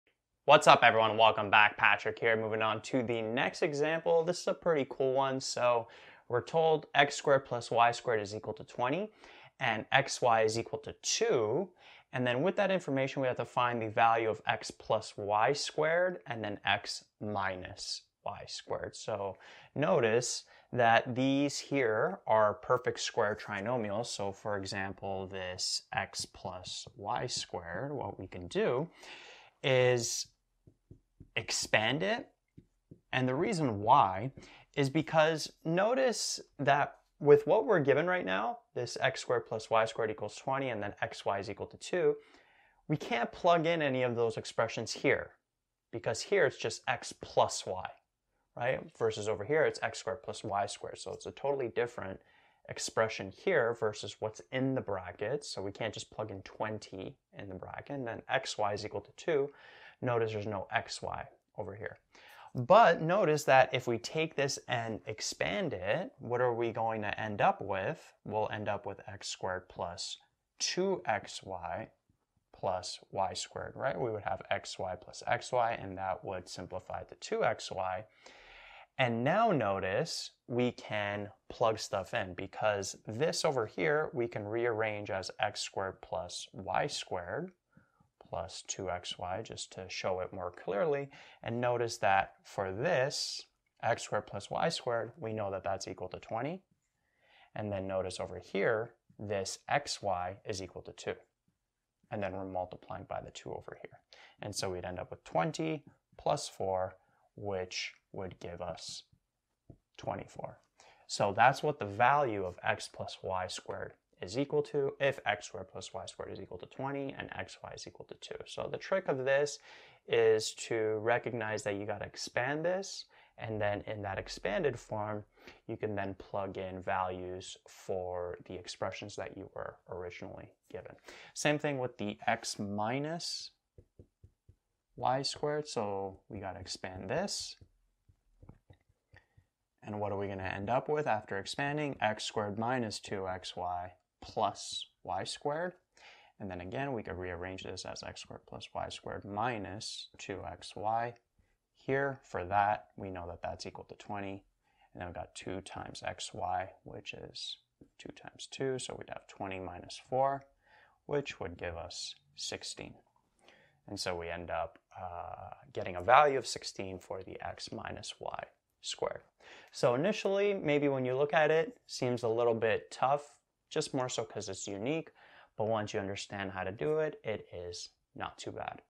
Grade 10 Math (Ontario, MPM2D) - Finding the Value of Factored Expressions
MPM1D Grade 9 Academic Math (Academic)